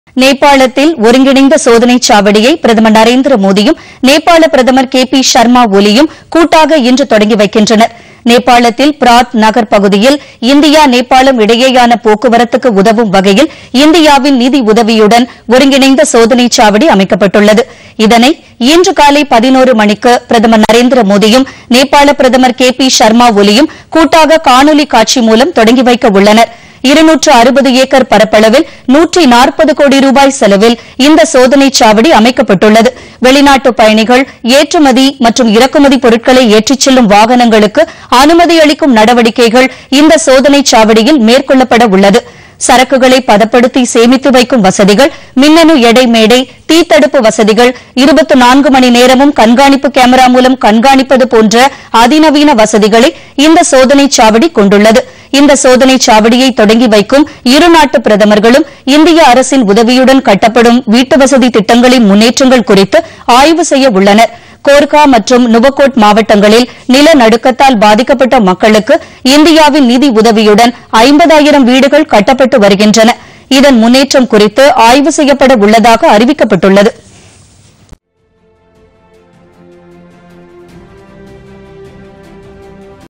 ஒருங்கிணைந்த சோதனைச்சாவடி இன்று திறப்பு
நேபாளத்தில் ஒருங்கிணைந்த சோதனைச்சாவடியை பிரதமர் நரேந்திர மோதி, நேபாள பிரதமர் கே பி சர்மா ஒலியும் இணைந்து இன்று தொடங்கி வைக்கின்றனர்.